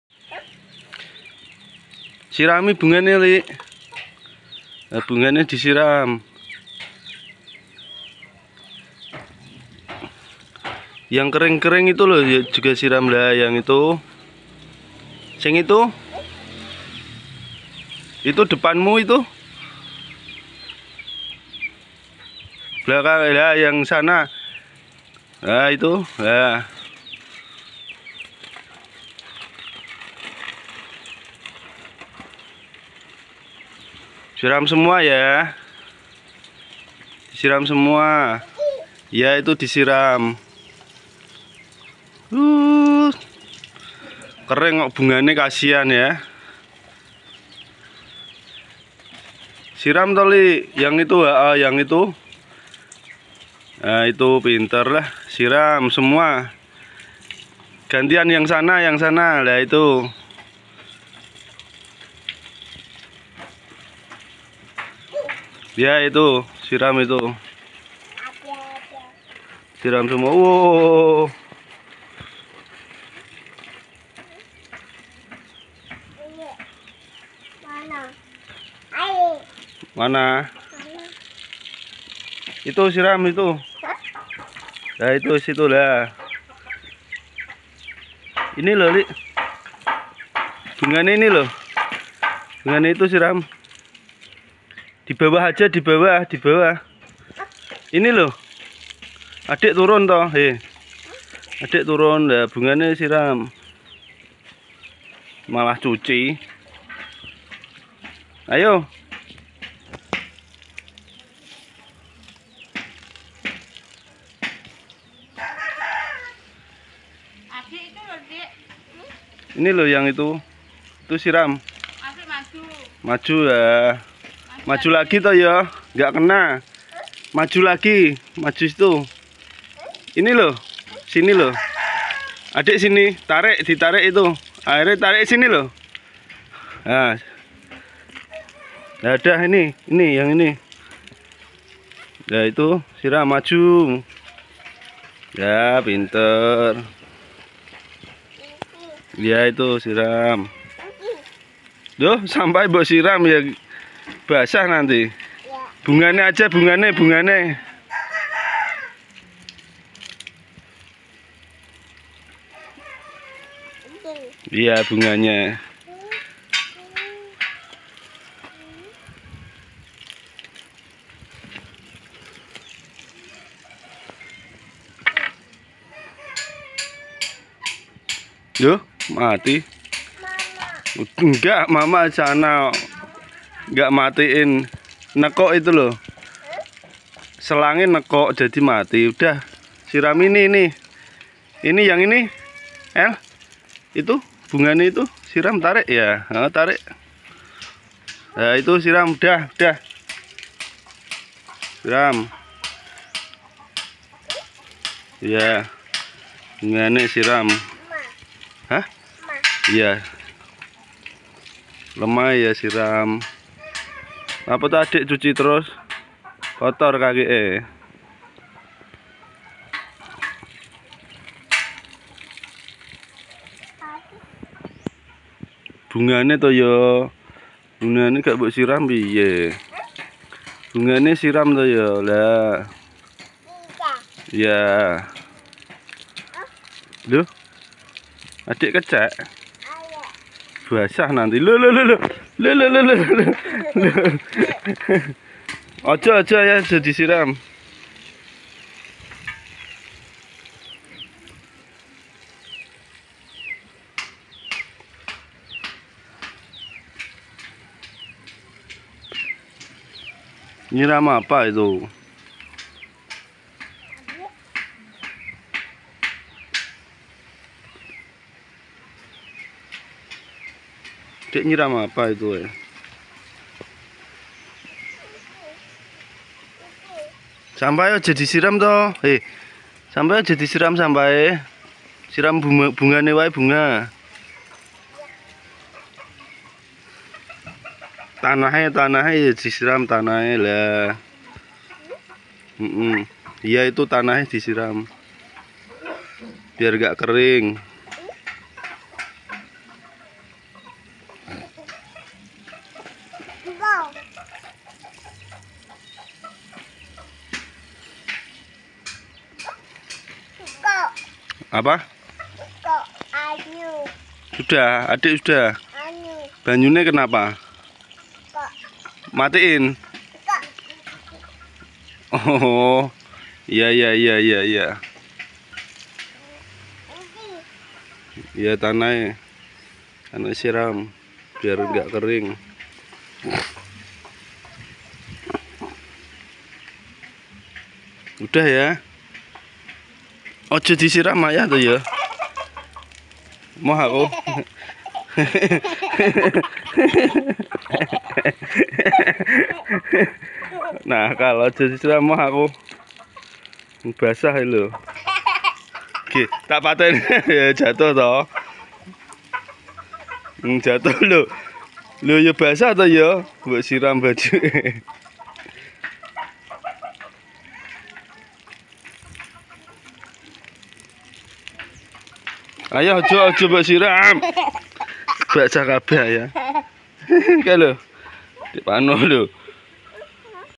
dek Elga lagi menyirami tanaman
halo teman teman dek Elga baru menyirami tanaman di depan rumah